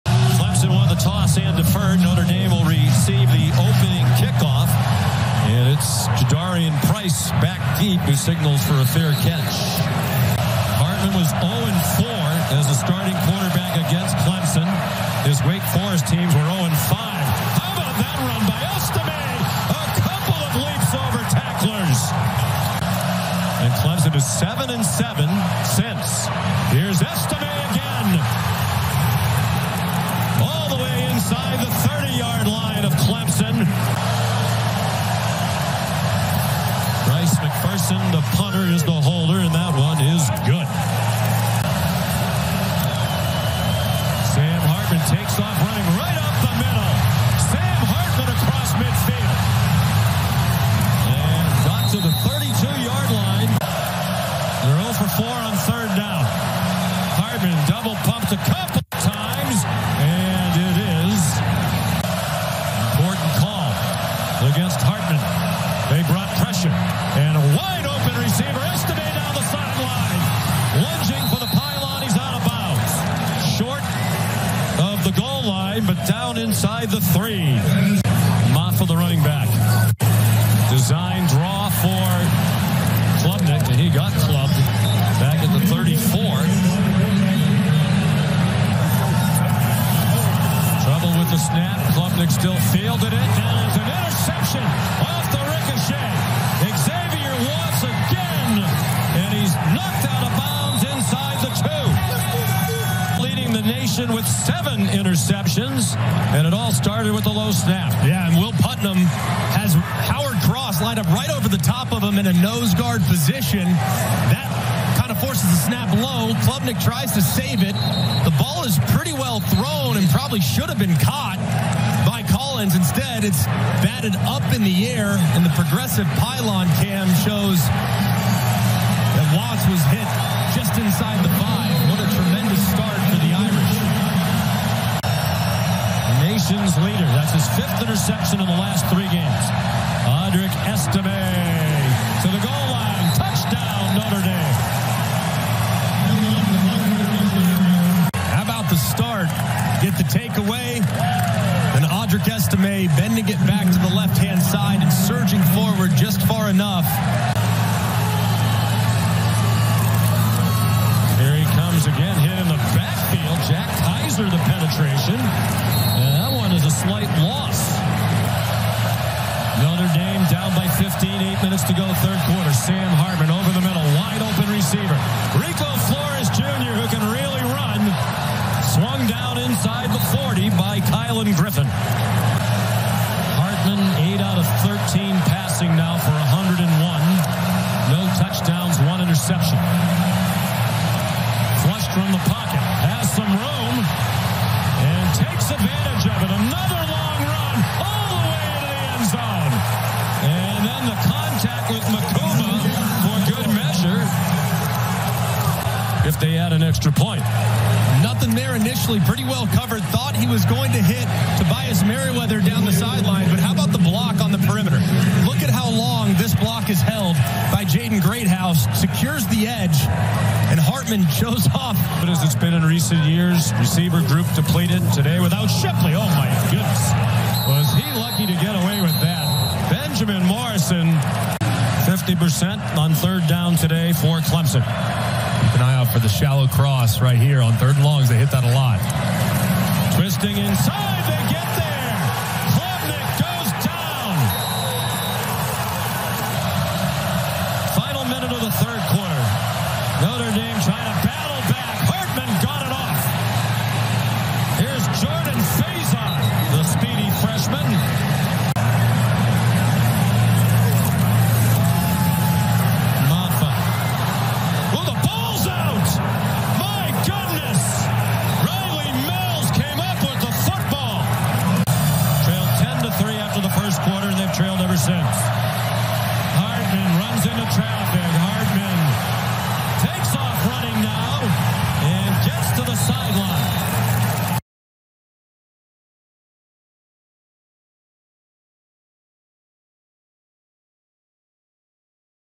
Irish Rally Falls Short Against Tigers | Highlights vs Clemson | Notre Dame Football
The No. 12/12 University of Notre Dame football team’s (7-3) second-half rally fell short on Saturday afternoon inside Memorial Stadium, falling to Clemson (5-4) by the score of 31-23. The Irish rallied down 24-9 at the half to cut it to one possession to start the fourth, but the Tigers special teams won the battle of field position, pinning the Notre Dame offense within its own 10-yard line on three occasions.

Notre Dame won the turnover battle 3-2 and produced more total yards, 329 to Clemson’s 285, but ultimately struggled to find a rhythm offensively. Sam Hartman finished 13-of-30 for 146 yards with one touchdown and two interceptions, while also picking up 68 rushing yards on seven carries. Audric Estime was Notre Dame’s leading rusher and receiver on the day with 120 all-purpose yards, including one rushing touchdown.

Junior safety Xavier Watts continued his hot stretch of the season with his third straight game with an interception (five interceptions in that span) and nation-leading seventh overall. Watts also finished with three tackles.

Defensive lineman Rylie Mills notched eight total tackles and a key fumble recovery. Linebacker Jack Kiser led the defensive unit with nine total tackles on Saturday.